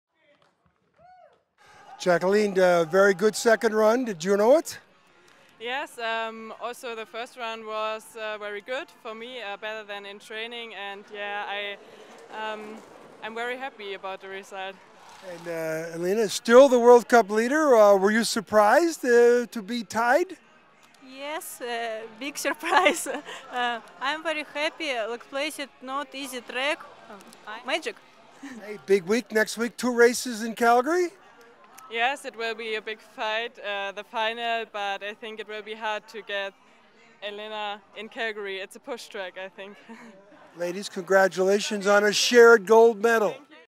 Lölling and Nikitina: "Will be a fight in Calgary" | IBSF Official
The win meant a third victory of the 2018/2019 World Cup winter for Elena Nikitina and her first in Lake Placid. Jacqueline Lölling is the first German female winner in Lake Placid since Marion Thees celebrated success in 2010/2011.